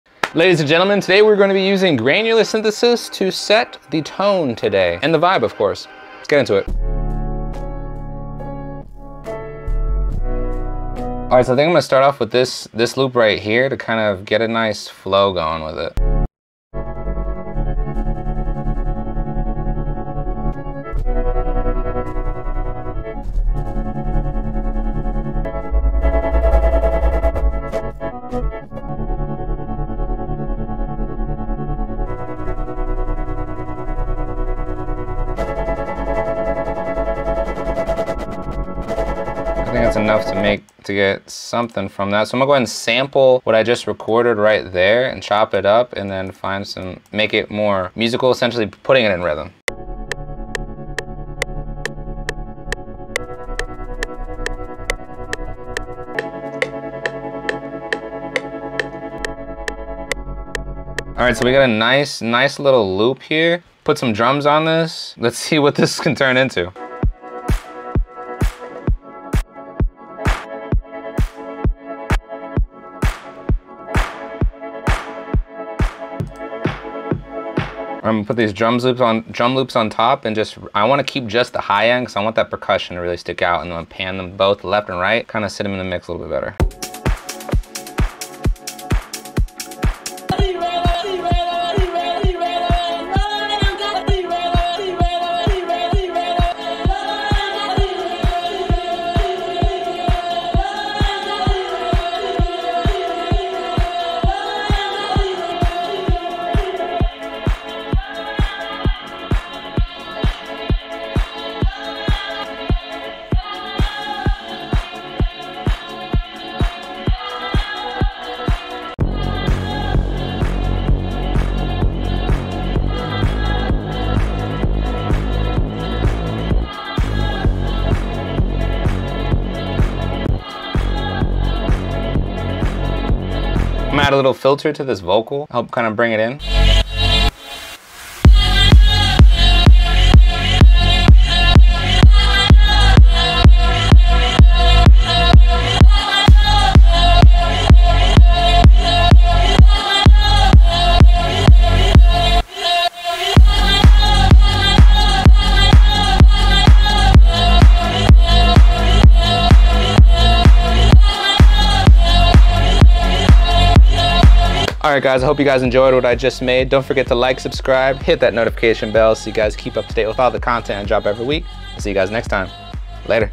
I Used Quanta Granular Synth To Make A Beat!
I've been trying to step out of my comfort zone lately. Let's see what the future will hold!

Follow Maycloud!